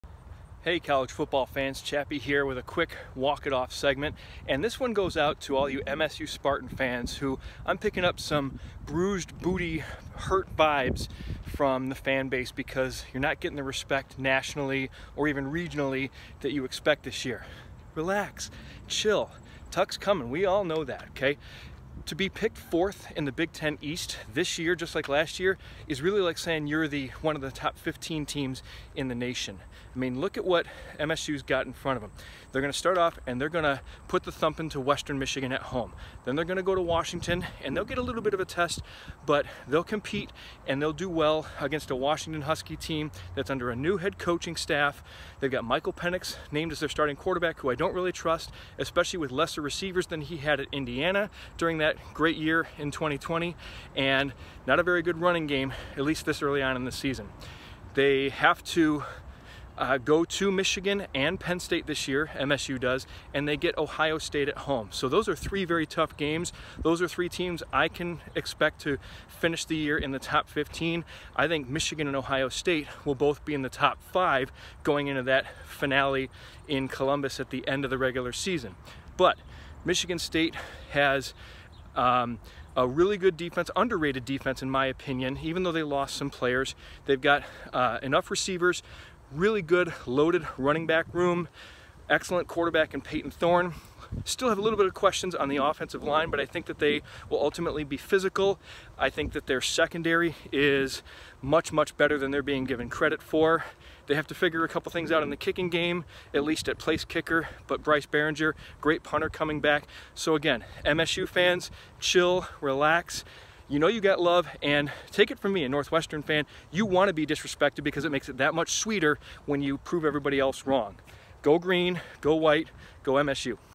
🤙🏈 “Walk it Off” with Chappy- Ep.65: MSU and Disrespect PART 1
Chappy tries to calm down MSU football fans who feel their team is being disrespected after a double-digit win success season last year. Embrace the struggle, and Sparty on! 👀👇